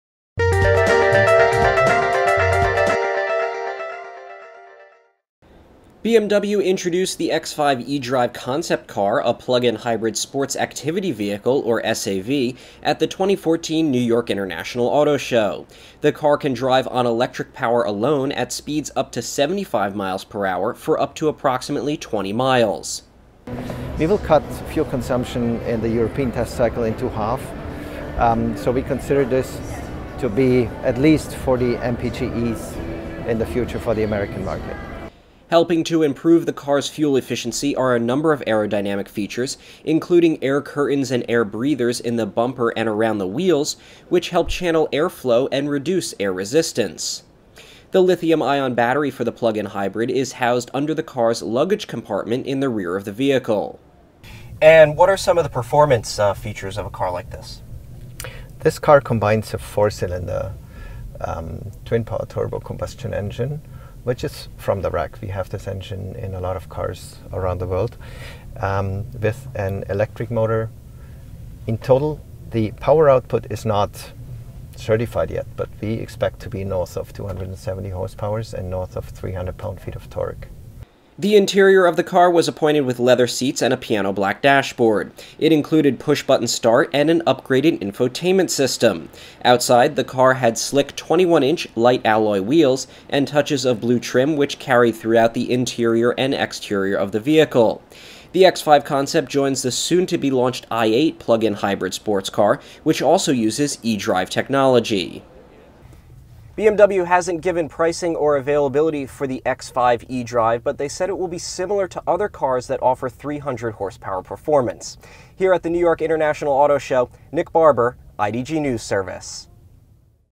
BMW's X5 eDrive plug in hybrid concept coming to US
BMW introduced the X5 eDrive concept car, a plug-in hybrid Sports Activity Vehicle or SAV, at the 2014 New York International Auto Show. Follow reporter Nick Barber on Twitter @nickjb